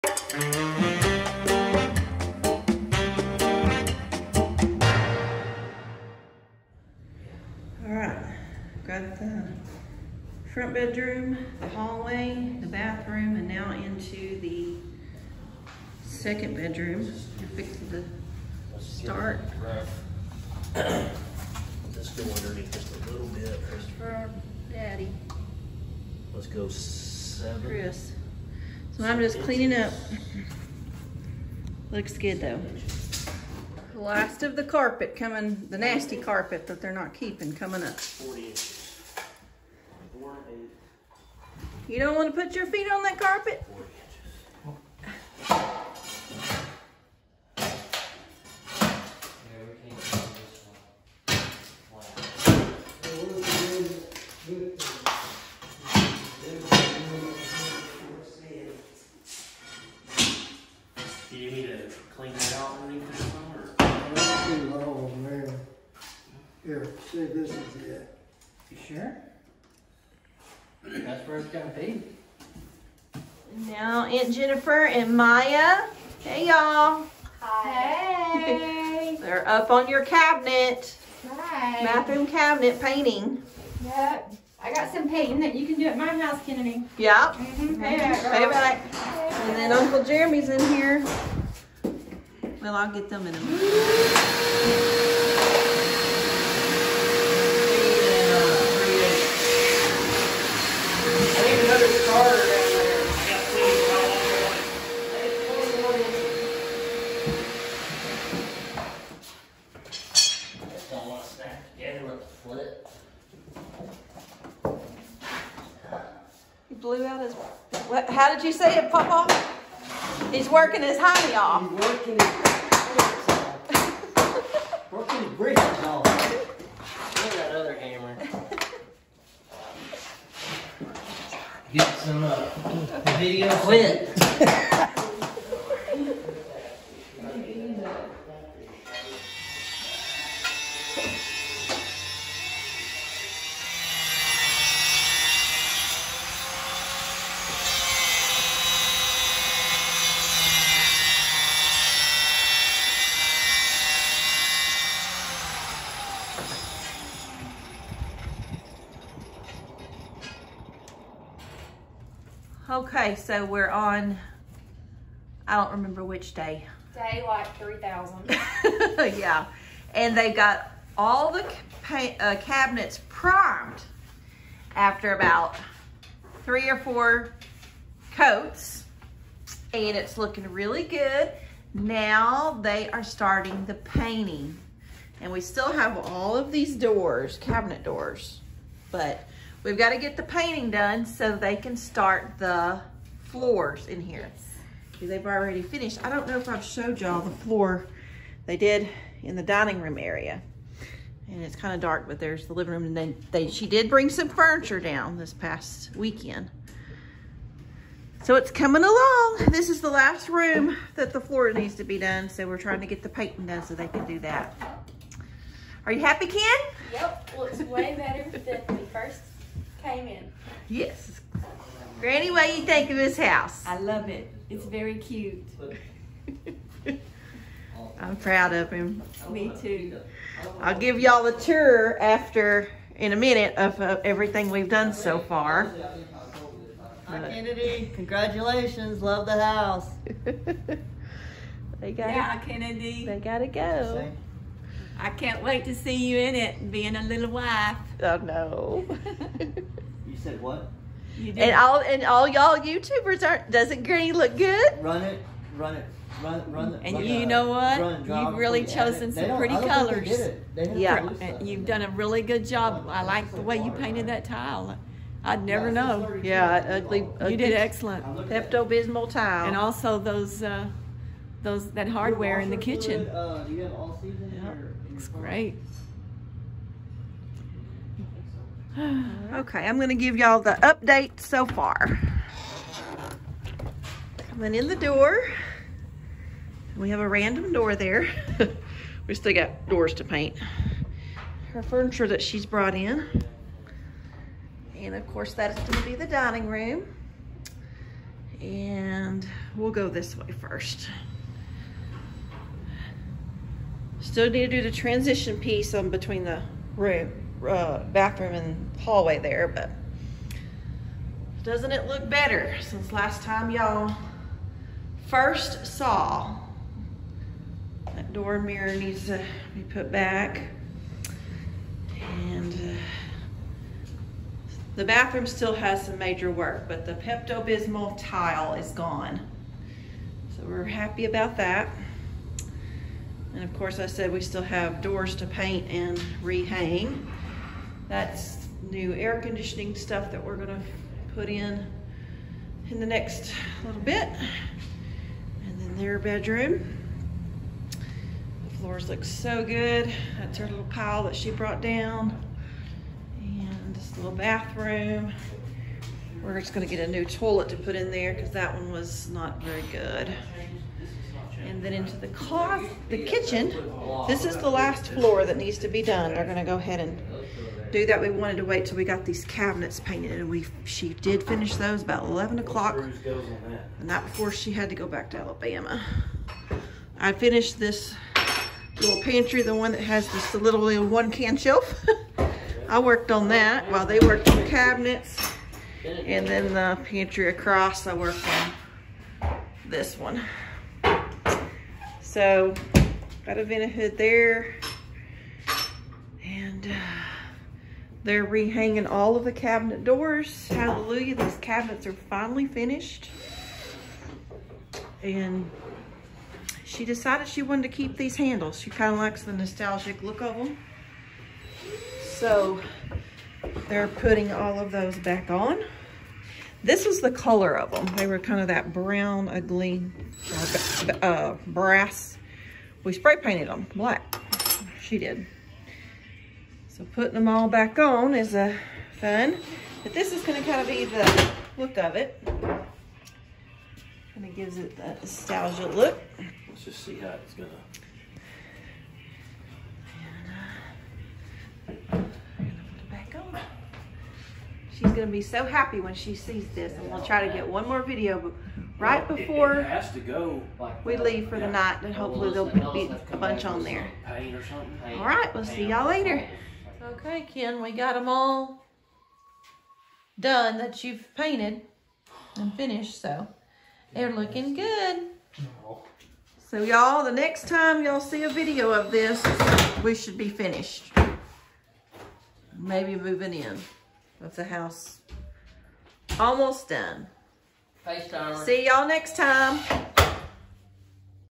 UPDATE ON THE KIDS HOUSE
Thankful for family that comes together to help out the kids start their life together.